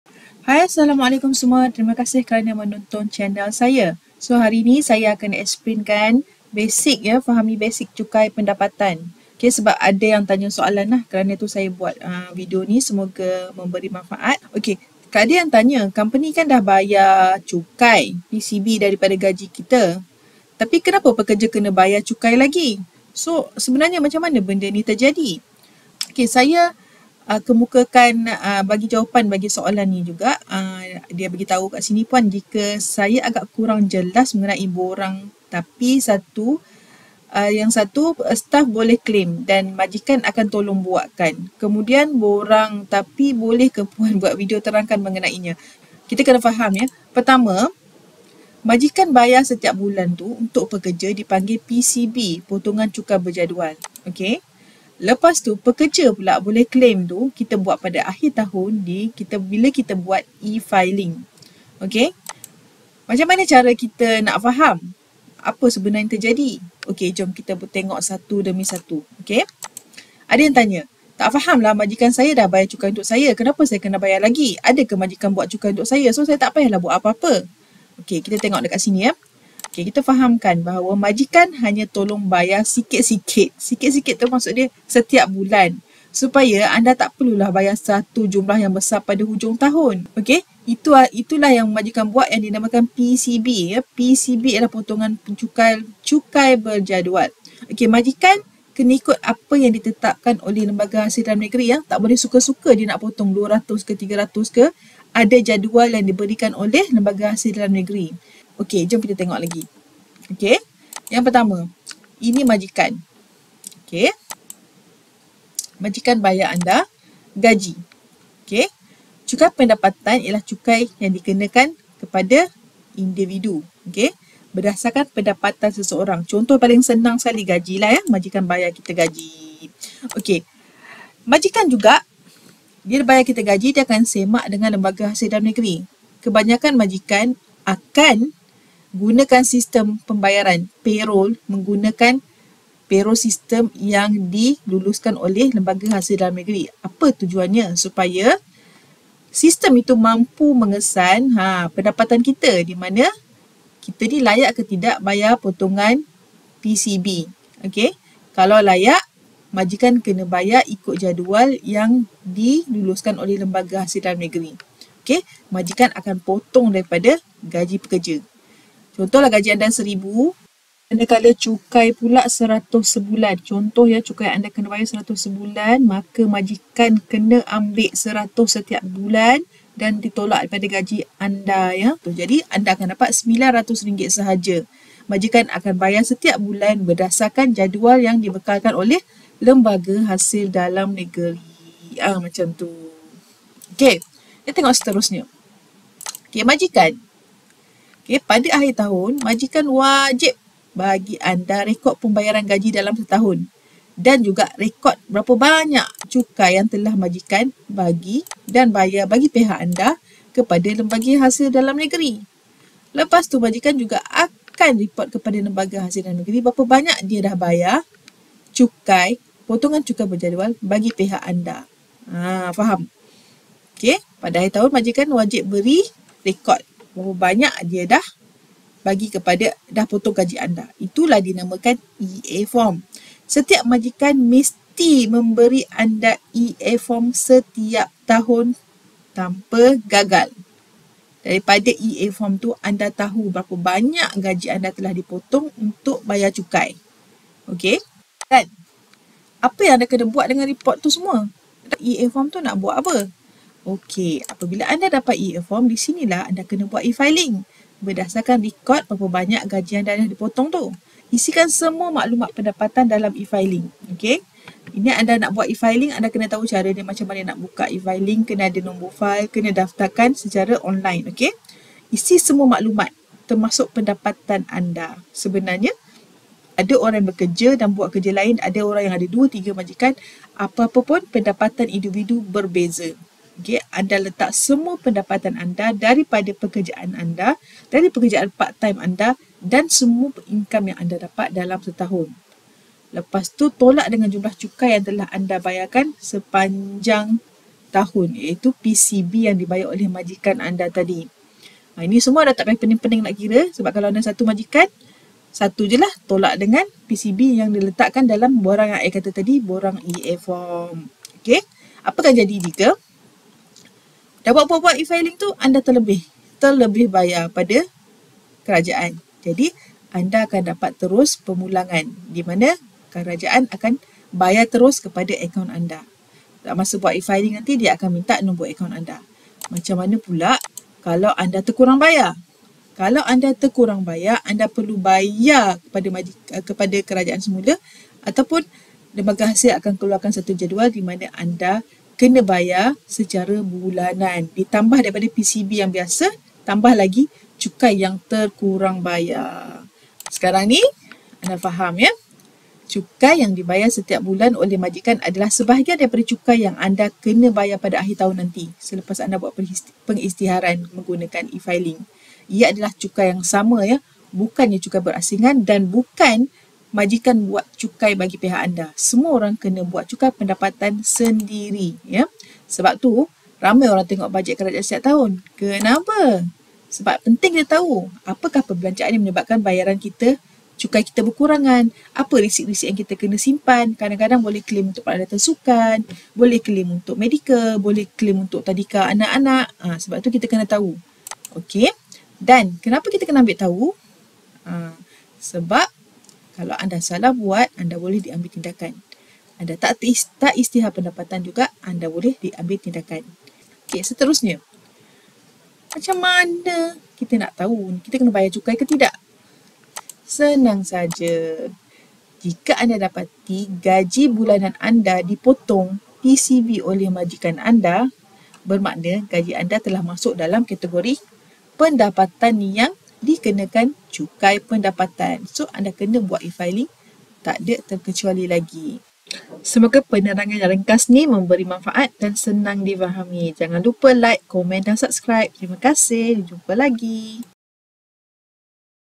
Kaitan Antara Pekerja, Cukai dan Majikan
Adakah majikan buat cukai untuk pekerja?
Apa perananan pekerja bila ada potongan PCB?
Kenapa say aken abayar cukai lagi? bukan majikan sudah bayar ke?
Jom kita faham basic cukai pendapatan